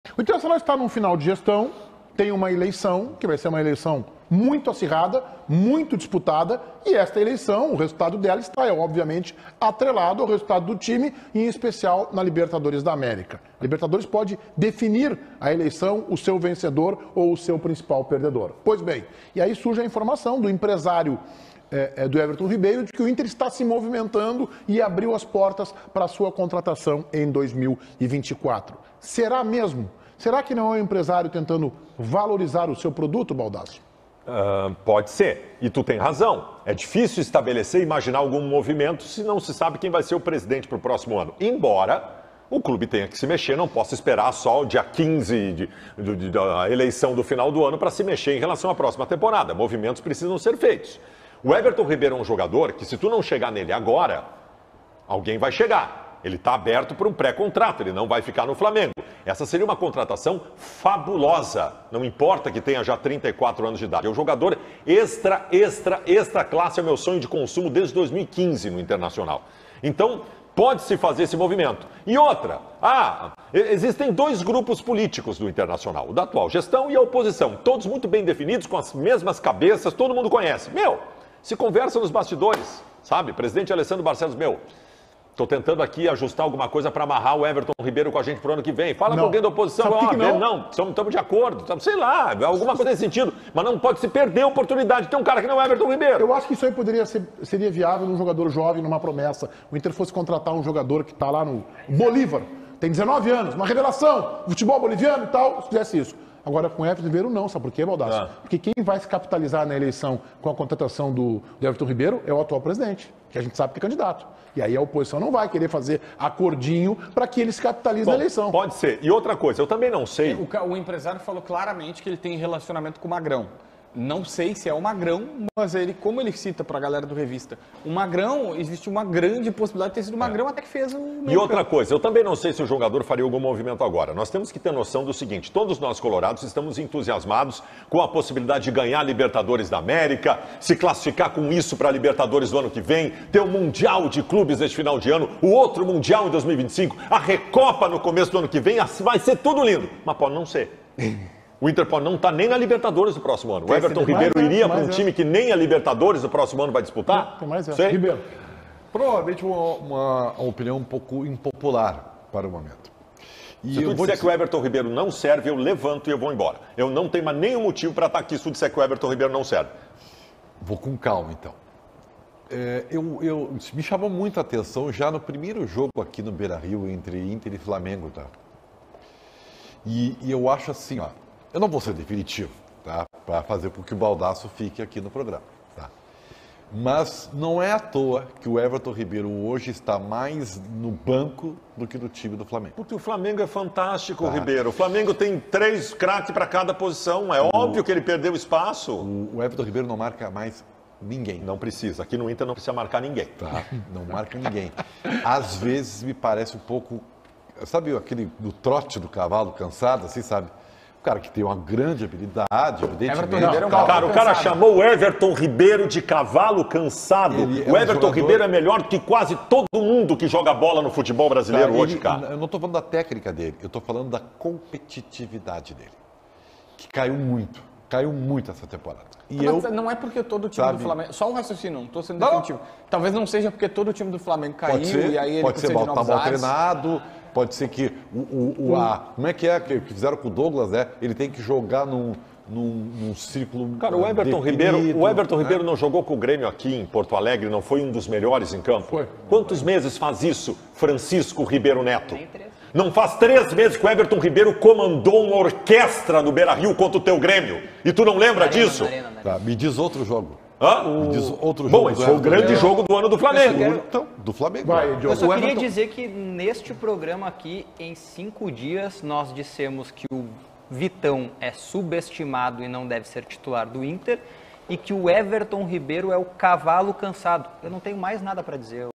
Inter monitora situação de Everton Ribeiro para 2024
O time do Inter está de olho no jogador do Flamengo, Everton Ribeiro, para o próximo ano. O atléta que não jogará no time carioca no próximo ano, seria uma ótima contratação para o time do Internacional.